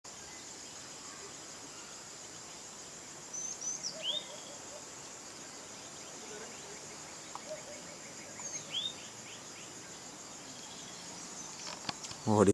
A beautiful British Coal Tit in Munnar
We have captured this video in Munnar. We stayed in a resort, there i have captured the videos of almost 10 new variety of birds.

The coal tit (Periparus ater) is a passerine bird in the tit family Paridae. It is a widespread and common resident breeder throughout temperate to subtropical Eurasia and northern Africa. The black-crested tit is now usually included in this species.